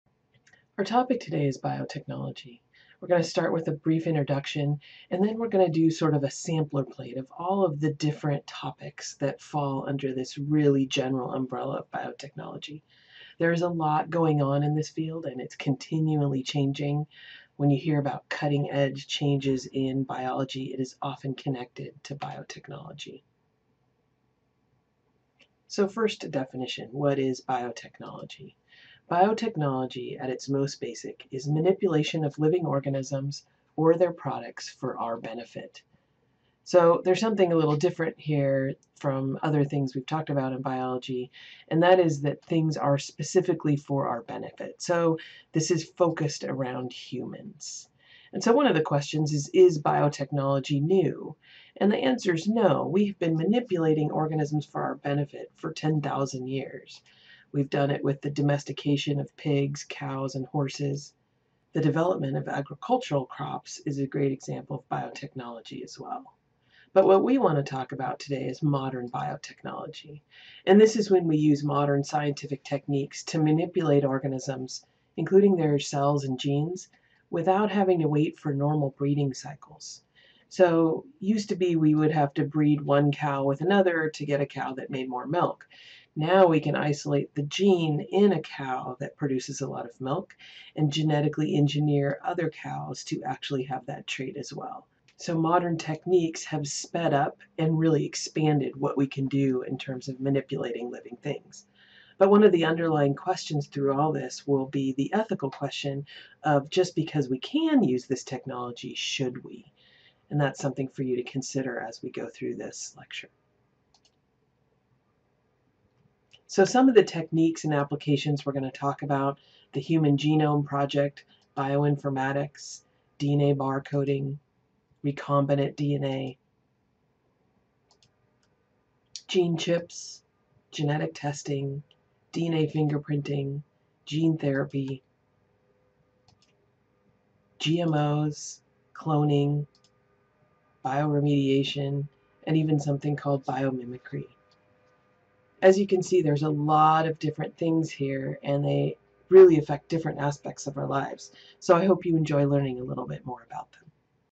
Biotechnology Lecture Video 1
Biology 300 lecture 1 by Susan Ramones - Intro to biotechnology